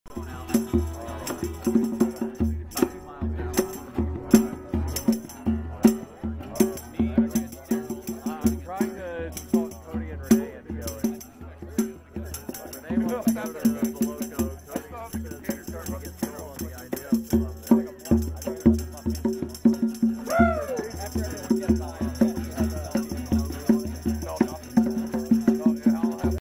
2016 04 15 Drums at NH Unity Cypher
Friday 15 April 2016 12:39pm
NH Capitol Plaza - Concord NH
Musical performances at the Unity Cypher torch lighting and cannabis legalization rally on April 15, 2016.
Filmed using a Nikon Coolpix P520 by Garret Ean